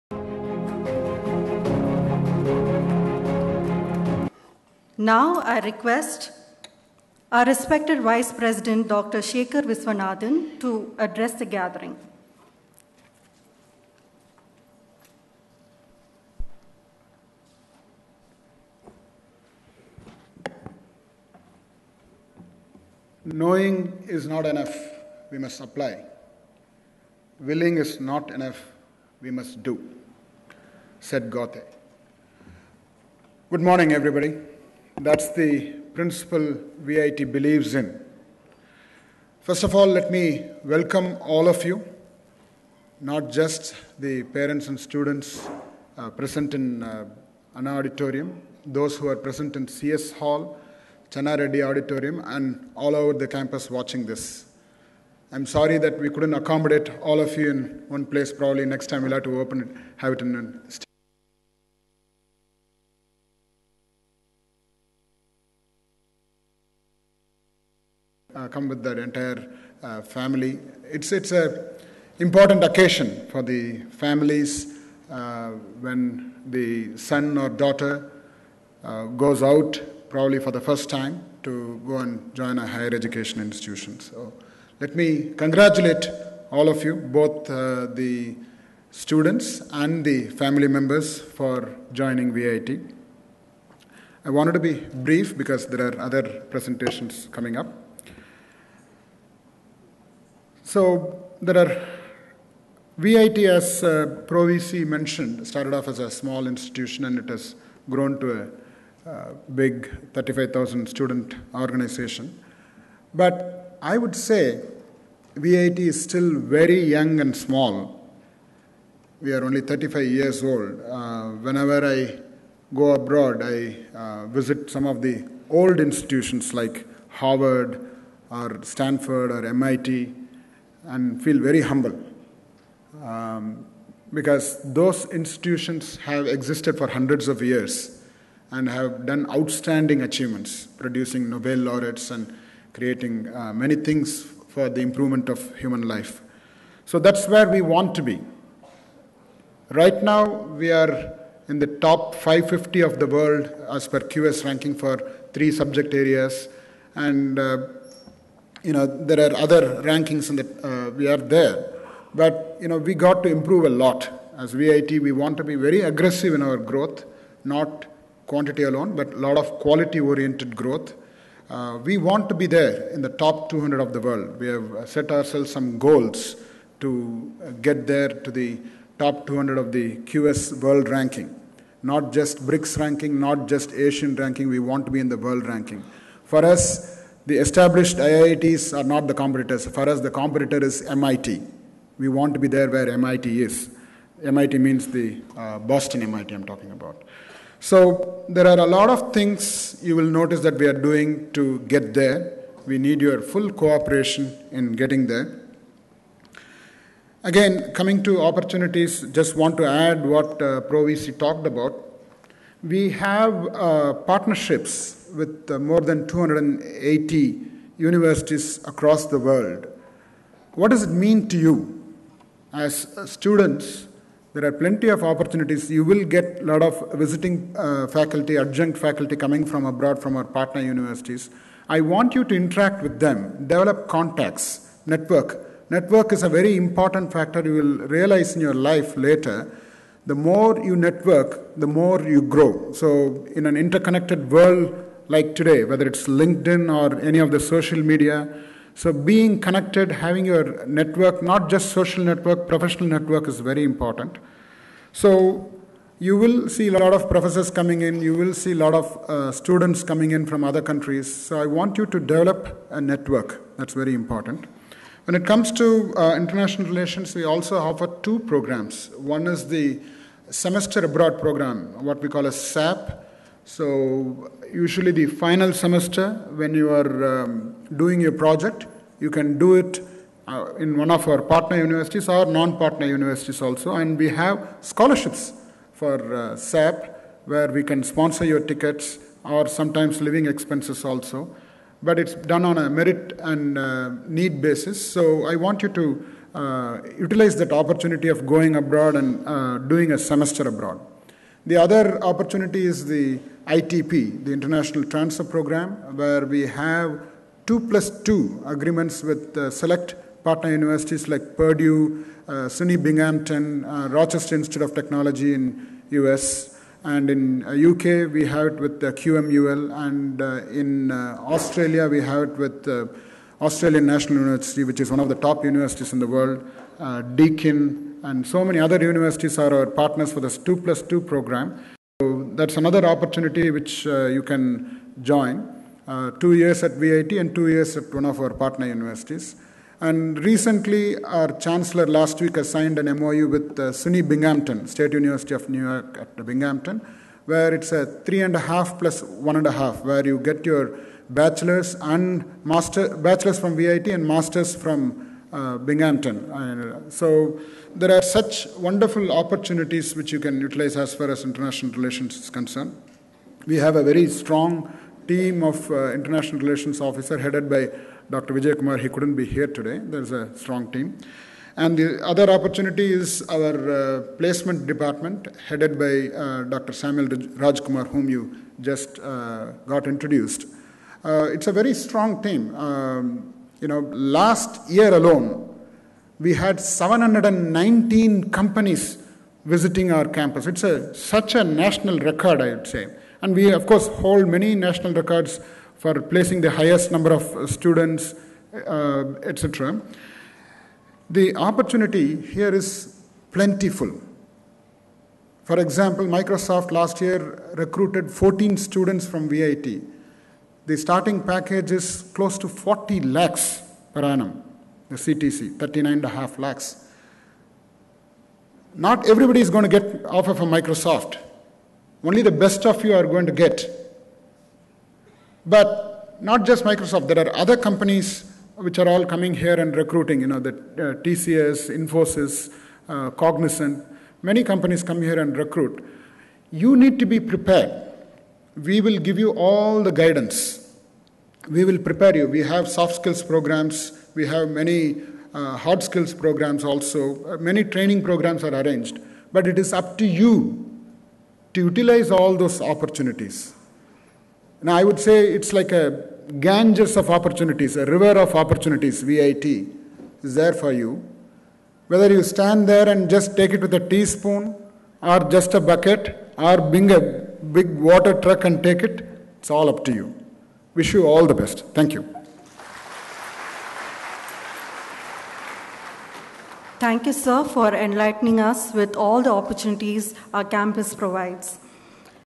B.Tech Induction Programme Session 1 - 2019
It's hard to introduce Sekar without talking about VIT. He was a student of VIT, and first served the institution in the 90s as Vice-Chairman. He contributed to the growth of the University from its humble beginnings as Vellore Engineering College. His leadership was instrumental in the founding years of the institution, and in preparing VEC to become VIT University.

Sekar is a rank holder in Electronics and Communication Engineering from University of Madras. He received his Master's degree in Computer Science from Rochester Institute of Technology, Rochester, USA.

During his leadership as Vice-Chairman of VEC, he introduced various additional branches including Computer Science, IT, Chemical and EEE. He diversified beyond engineering by starting MBA and MCA programmes. Mr.Sekar envisioned autonomy for the institute so that it can excel in curriculum, start new programmes and set high standards in education. He lead the institution in obtaining ISO certification, NBA accreditation and preparation to become a deemed university. His contributions paved the way for VEC to become a University.

Between his tenures at the institution, Sekar worked in the USA to get more experience from the wider world. He worked at IBM, NetClerk and Walmart.com. Walmart.com, USA conferred the "Best Engineering Manager" and "Best Achiever" awards. Sekar continuously tries to bring the best in the industry to the education world.

Sekar is currently the Vice President of VIT. He focuses on the university growth strategy, academic policies, international relations, public relations and alumni relations. He has brought in numerous improvements to benefit the students including STARS (Support the Advancement of Rural Students) scheme, Fully Flexible Credit System (FFCS) and the Semester Abroad Programme.  VIT is recognized as one of the top engineering institutions in the country today, with a sprawling green campus of 18,500 students from around the world.

Mr.Sekar was bestowed the prestigious Young EDGE Award on 13 March 2012 in recognition of his innovative and useful contributions to the field of education. The Award is given to young academicians or academic administrators in recognition of their significant contributions to enhancing the quality of education and making a lasting impression in the field. Mr.Sekar received the award for initiating and implementing two of his pet projects , the STARS Scheme and the FFCS which  have helped numerous students.

Mr.Sekar continues to lead the University towards realizing the vision of the Chancellor to put VIT among the top universities of the world.